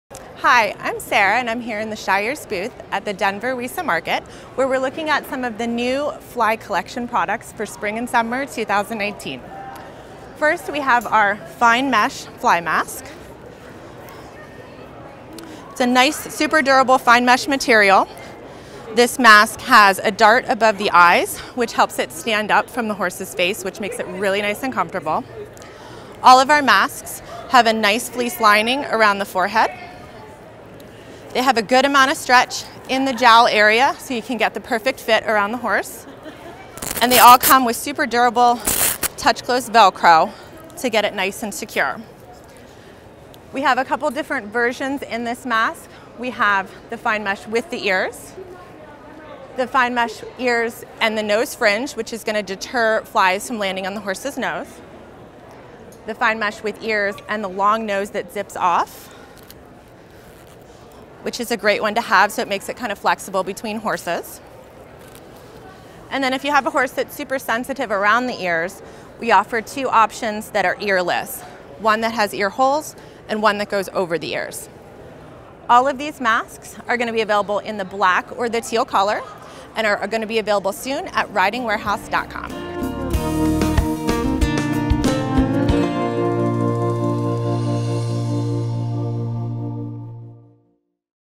Shires 2019 Fly Masks with Riding Warehouse | WESA 2019
For the first time, Riding Warehouse has been invited to film at the International Western/English Apparel & Equipment Market! We are so excited for the opportunity to show you all the new 2019 products from your favorite brands!